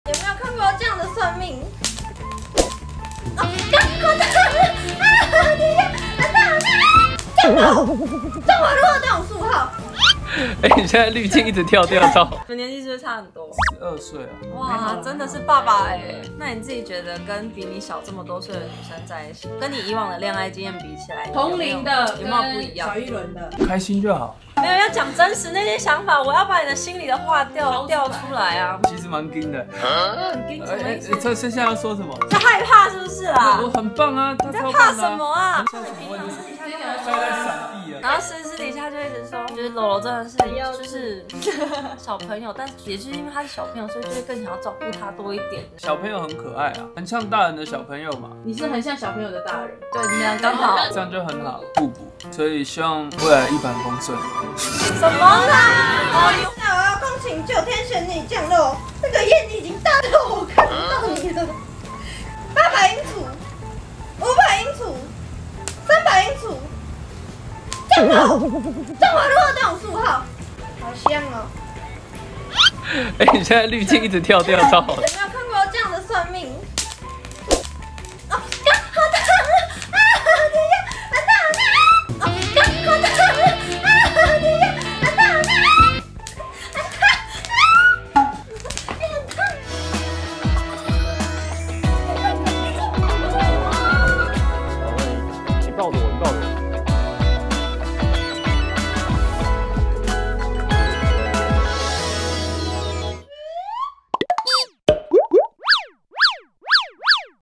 【酷炫女友】模仿「阿翰九天玄女」 竟降肉到該邊！痛到出鏡超爆笑
直接大翻車！感覺真的好痛XD

影片授權：TikTok/@0207celine、IG/yuchinjou